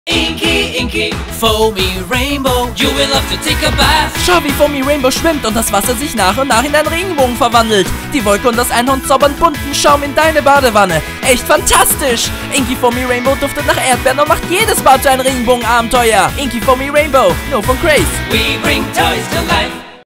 CRAZE Inkee Foamy Rainbow TV Commercial 2020 (deutsch) 2020
Diese Badebombe zaubert einen Regenbogen in die Badewanne!

Von Dermatest mit "sehr gut" bewertet und frei von Mikroplastik garantiert diese neue Badebombe sicheren Badespaß.